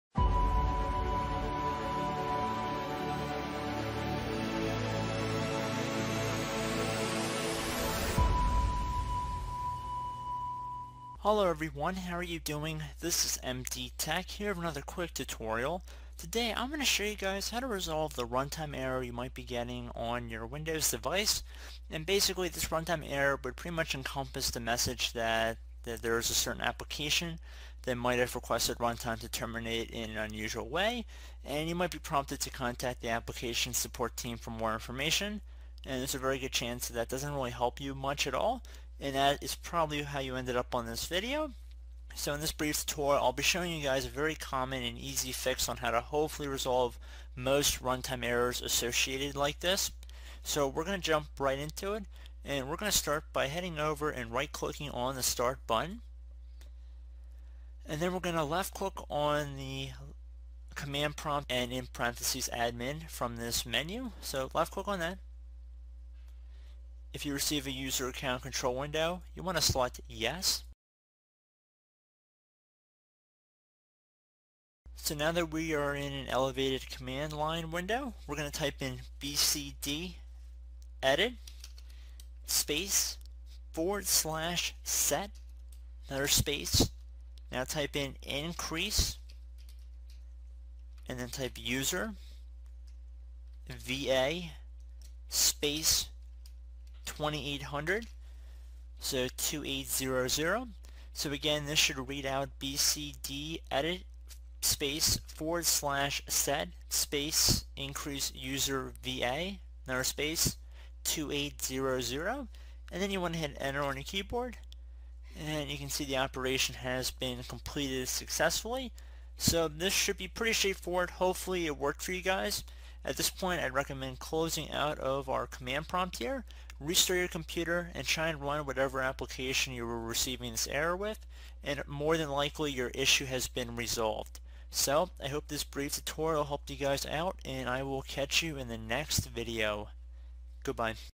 How to Fix All Microsoft Visual C++ Runtime Library Errors
Some Windows 10 users and a few Windows 8 users have reported an error involving Microsoft Visual C ++ Runtime Library. When Windows Explorer is opened, a runtime error occured randomly. This issue is caused by a faulty Visual C++ Runtime Library or certain applications which use the Runtime. To fix this issue, we’ll reinstall Visual C++ Runtime, run Windows updates, root out the culprit applications.

This issue is also carried over to Windows 10 Creators Update and many users are also getting the runtime errors on the Creators Update edition of Windows 10.